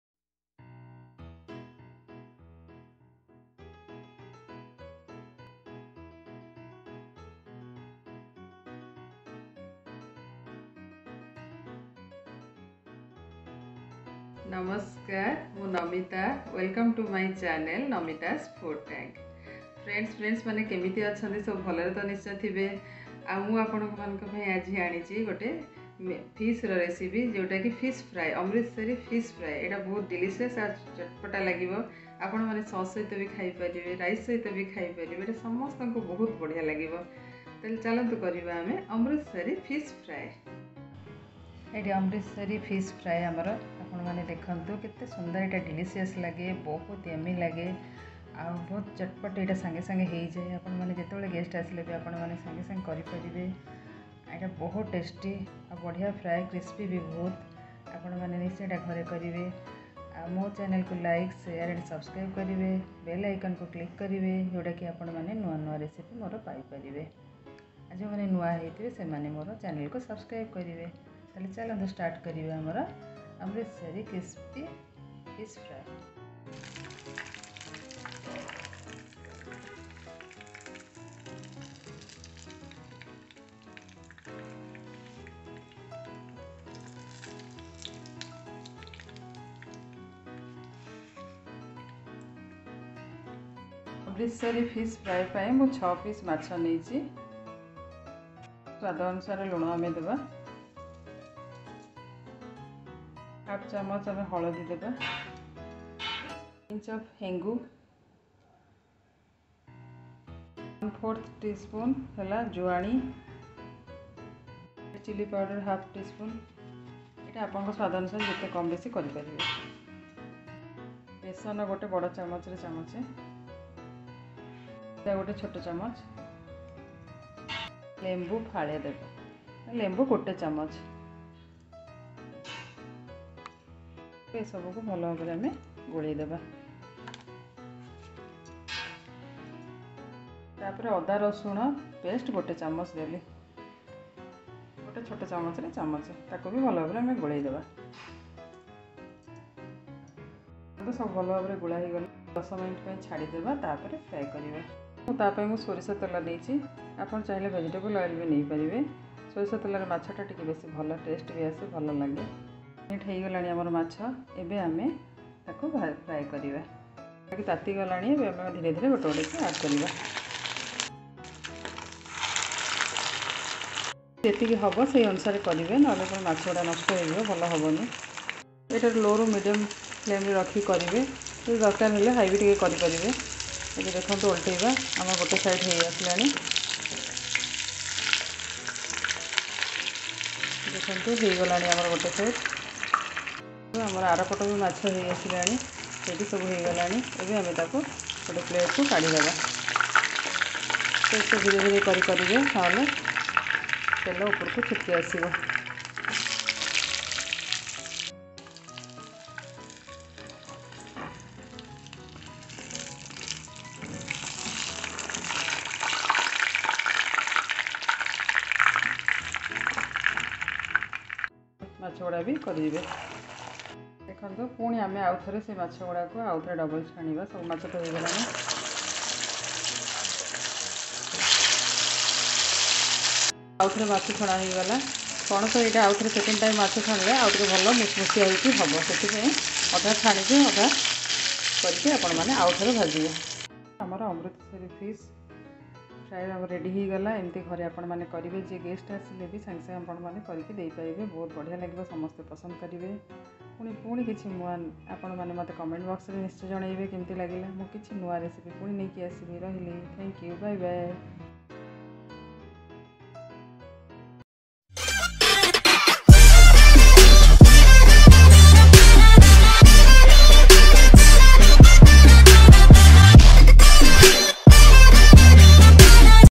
How To Make Amritsari Fish Fry/Easy & Chatpataa Fish Fry Recipe
Ingredients:
Fish-6pieces
Ajwain-1/4tsp
A pinch of hing
Mustard oil
Gram flour--1tbs
White flour-1tsp
Turmeric powder-1/2tsp
Red chilli powder-1/2tsp
Lemon juice-1tbs

Amritsarifishfry.